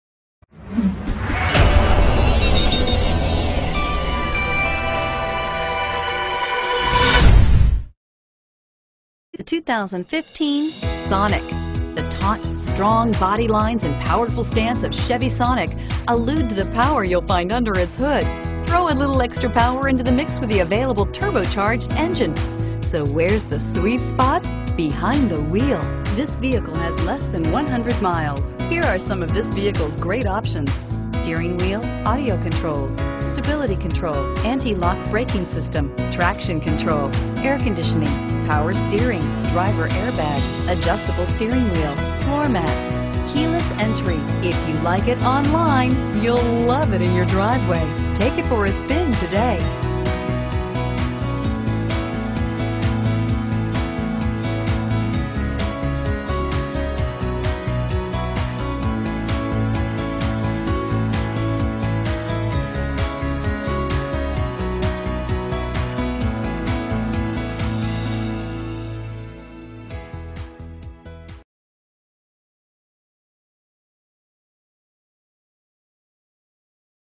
2015 Chevrolet Sonic Norman OK F4142215
2015 Chevrolet Sonic

Pick up this  2015 Chevrolet Sonic, available today at David Stanley Chevrolet of Norman. Featuring Automatic transmission, it has 6 miles. This could be the one you've been searching for.